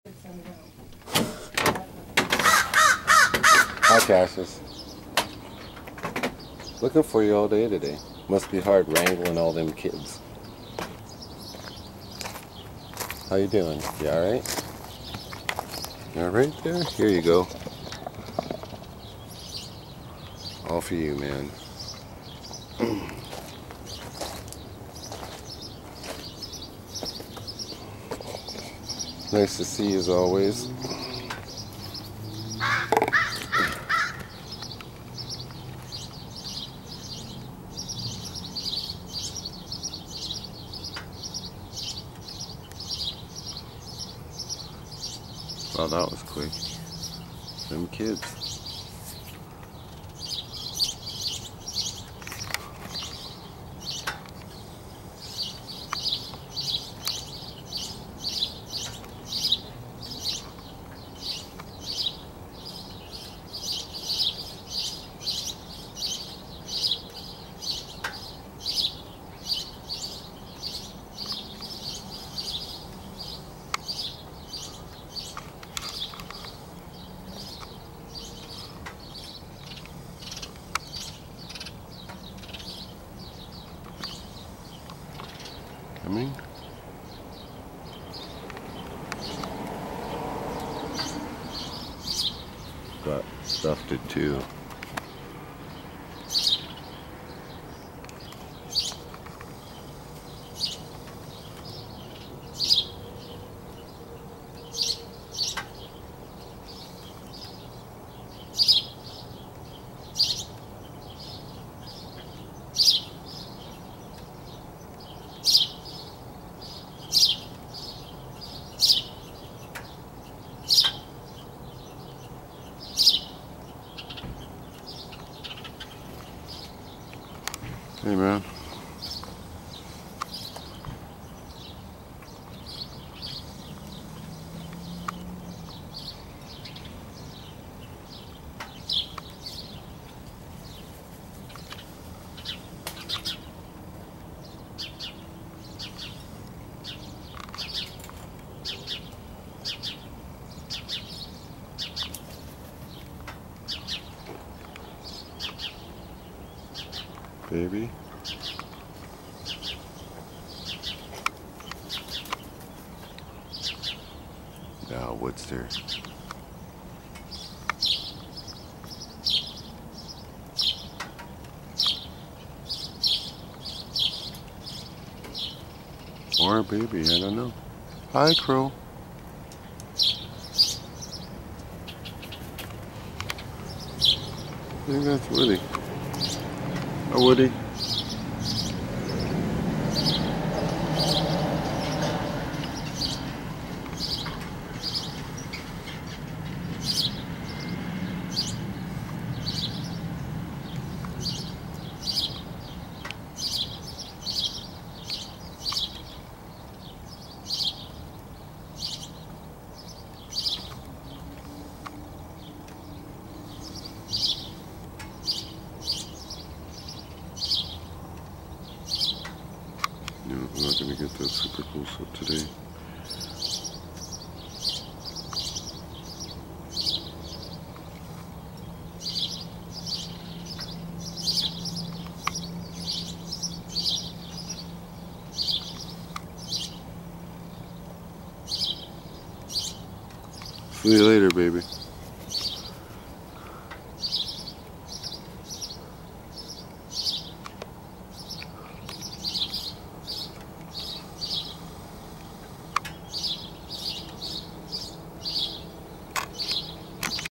6/27/2016 morning time w my birdie friends, crows and littles
Cassius Crow is back and forth at the beginning Woody Crow is at the end Littles inbetween